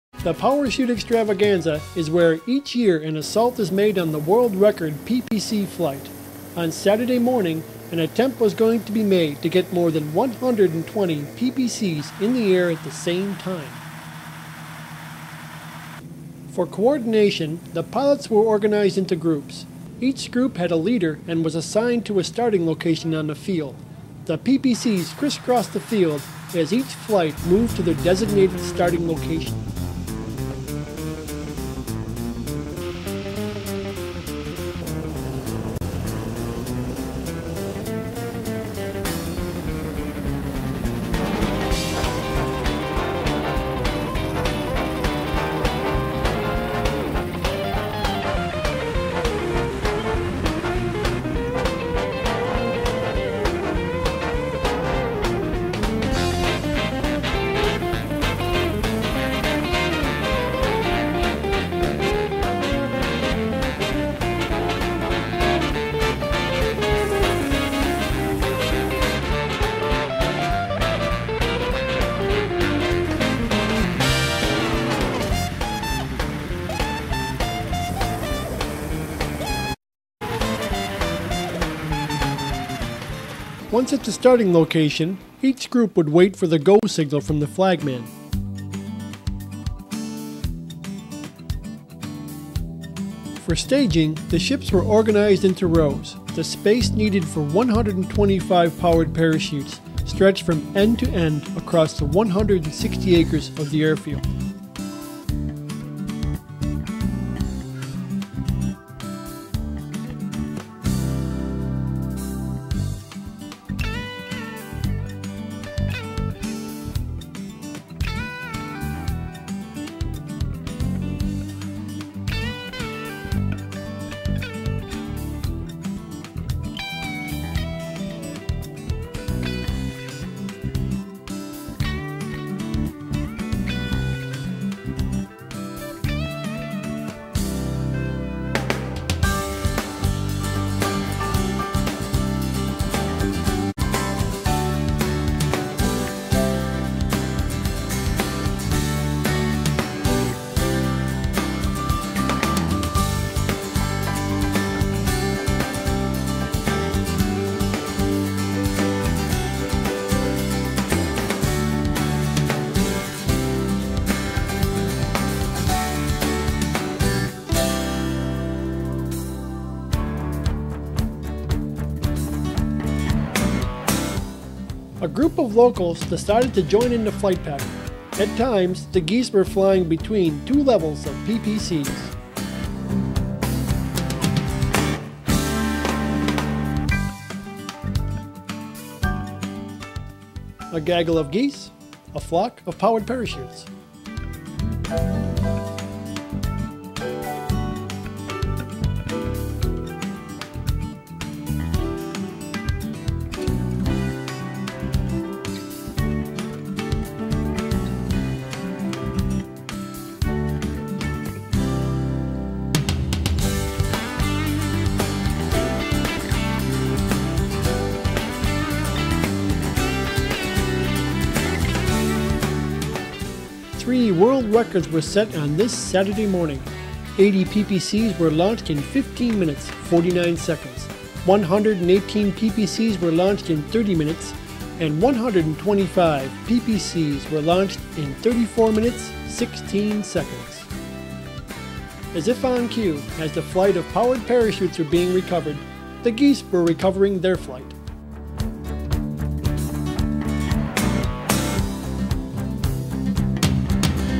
world record 100 ppc in the air
PowerChute fly-in had a world record 100 powered parachutes in the air